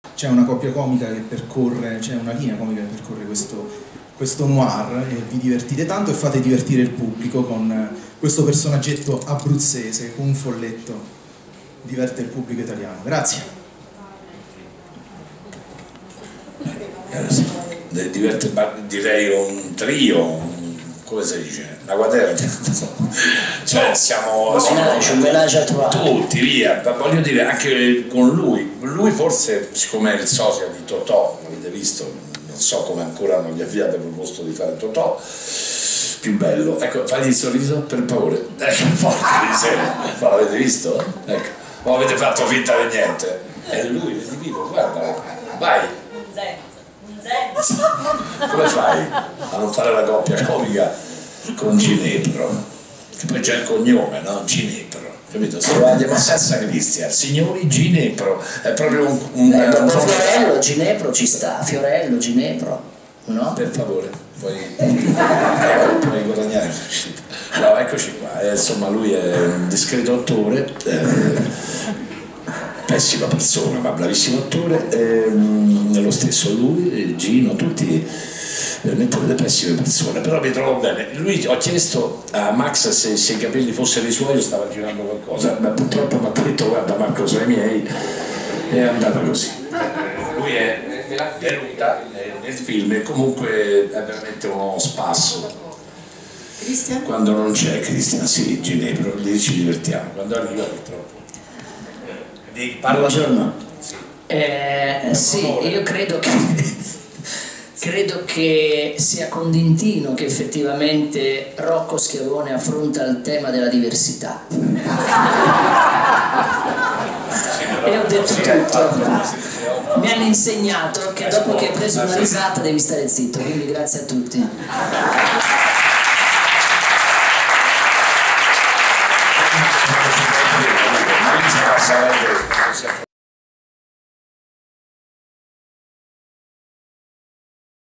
Rocco Schiavone 3, Marco Giallini e D'Intino (Christian Ginepro) scherzano
Alla presentazione di Rocco Schiavone, terza stagione, Marco Giallini e Christian Ginepro (attore che interpreta D'Intino) scherzano tra loro dimostrando un'ottima intesa anche fuori dal set (Video realizzato da Adriano Stabile).